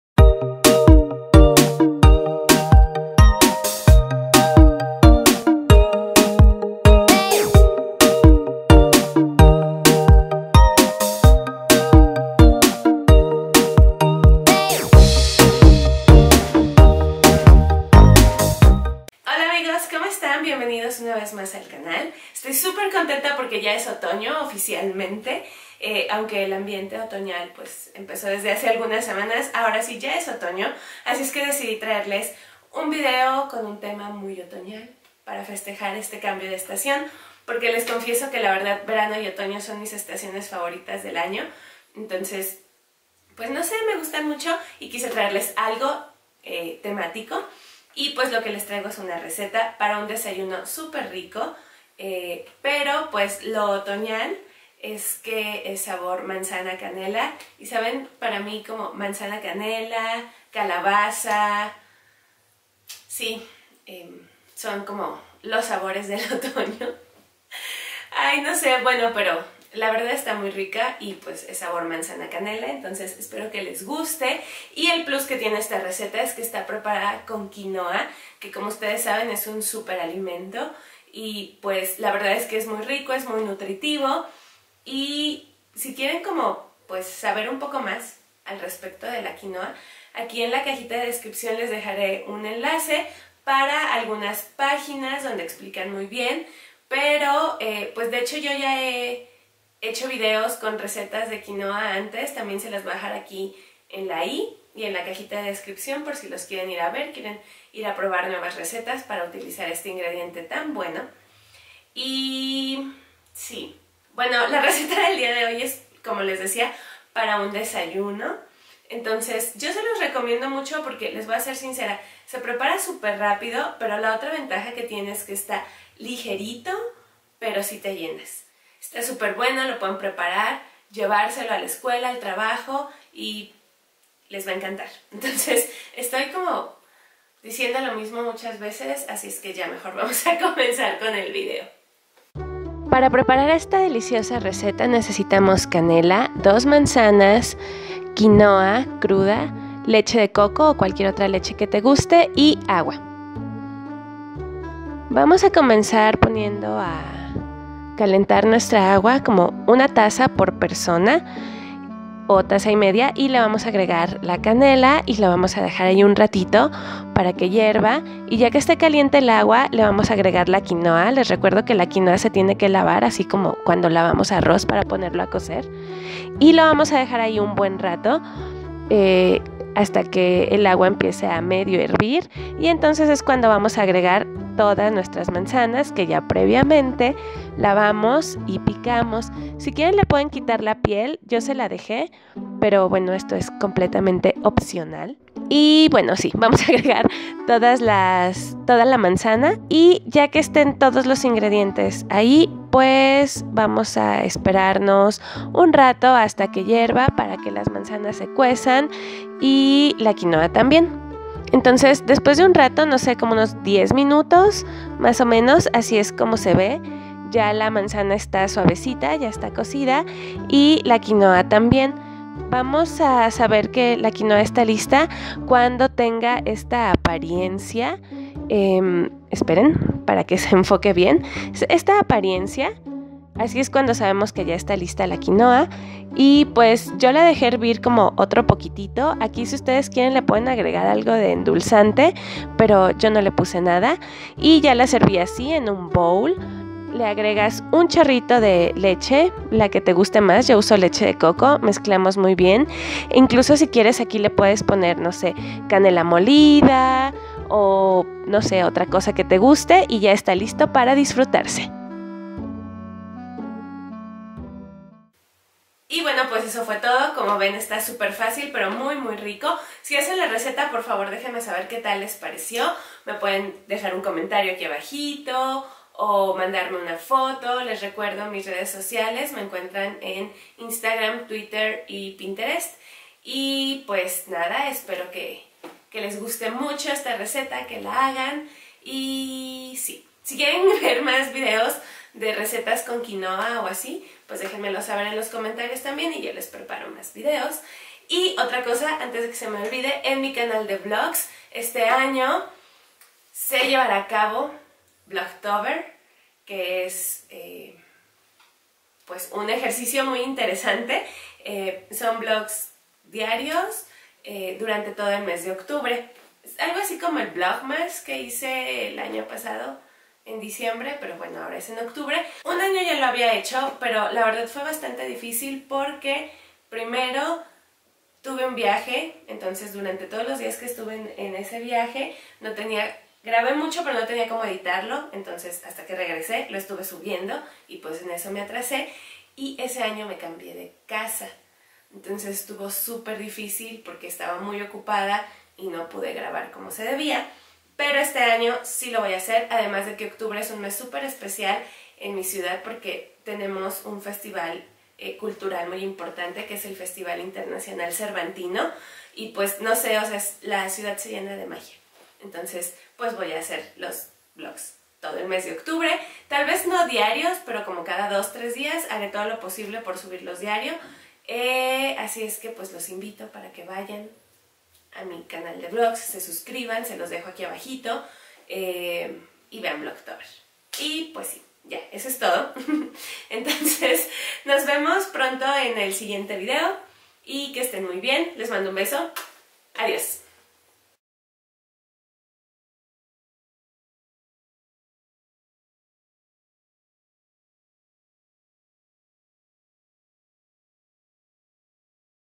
Pudín de quinoa y manzana
ღღ Despliega aquí para más información ღღ

El día de hoy les traigo una receta súper otoñal para preparar un delicioso desayuno de quinoa con manzana. ¡Que lo disfruten!